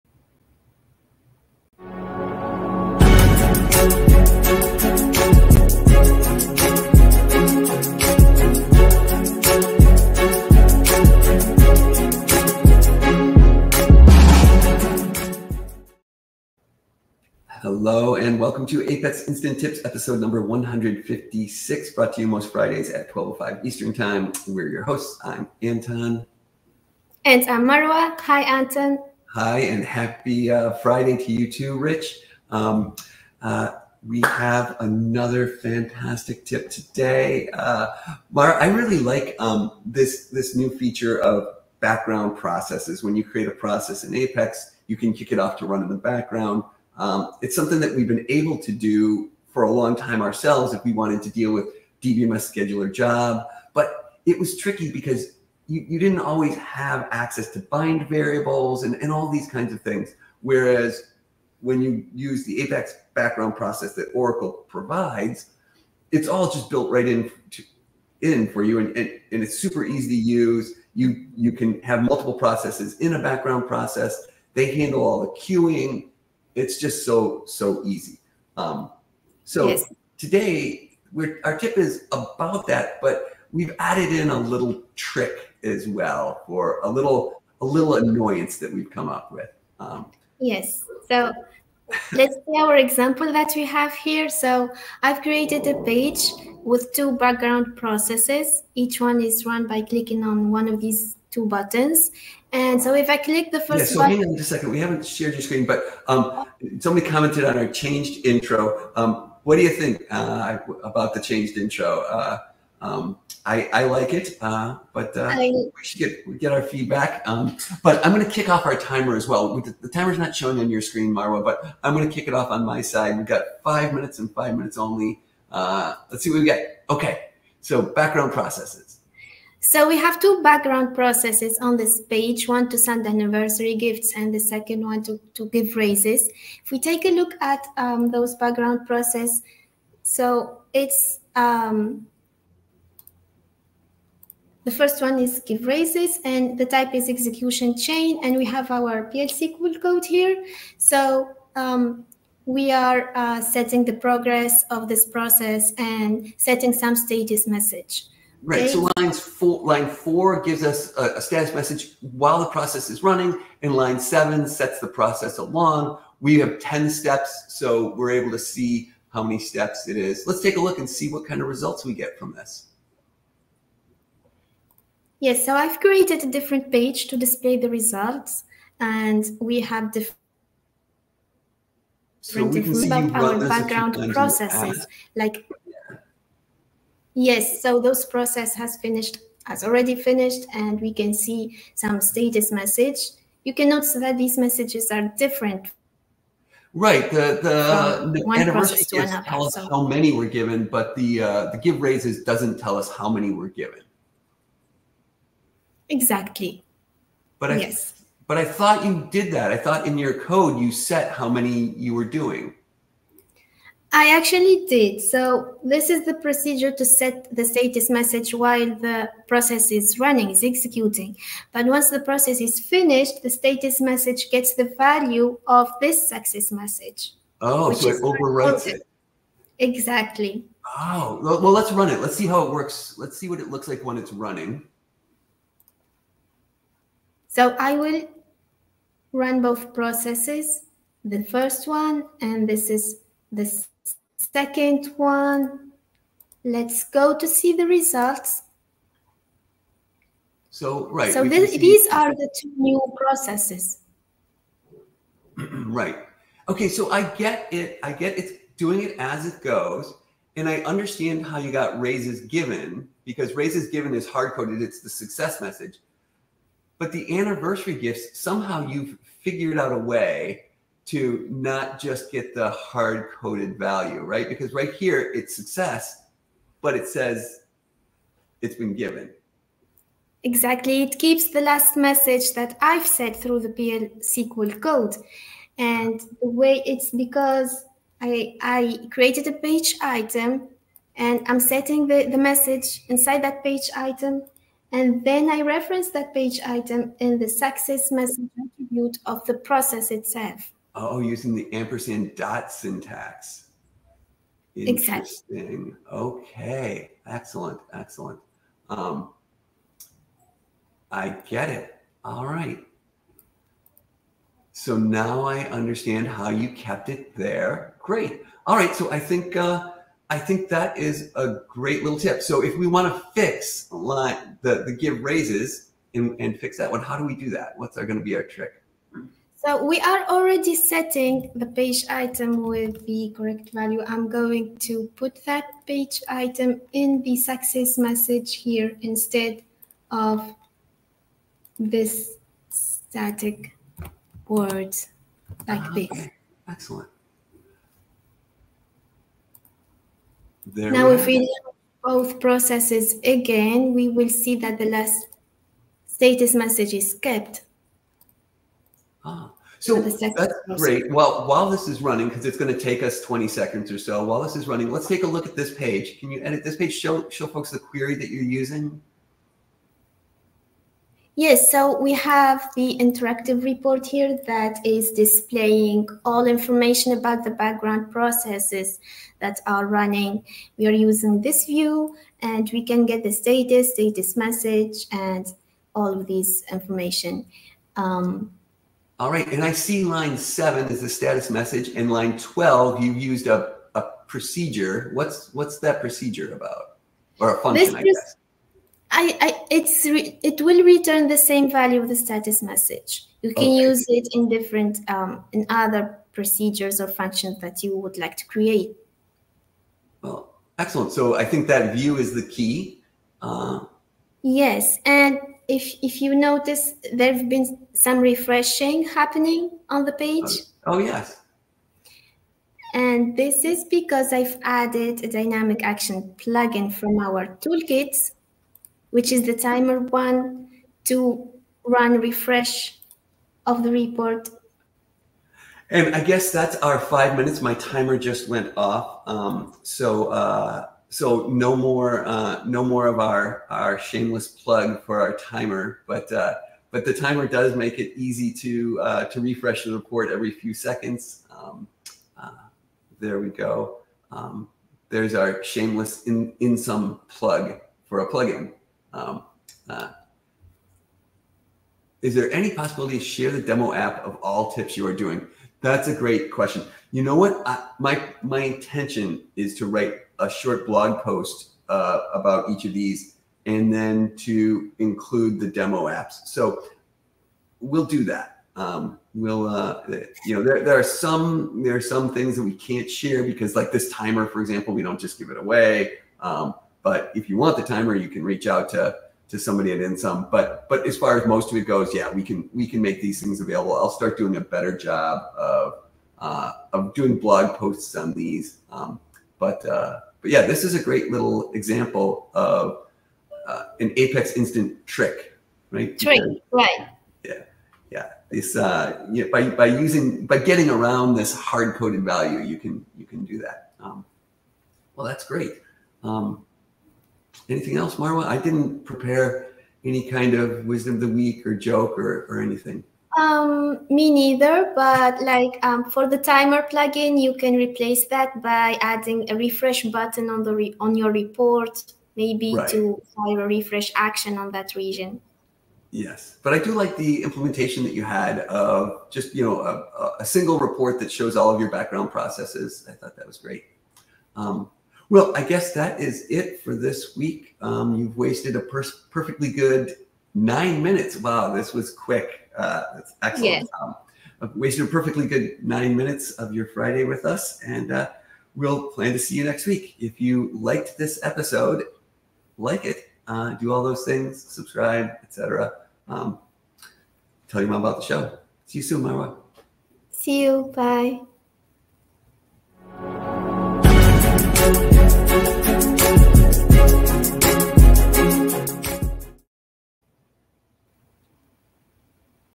APEX Instant Tips #156: Informing users about background processes
In this episode, Anton and Marwa discuss background processes: informing users of what is happening and (here's the tip) what happens upon the process's completion.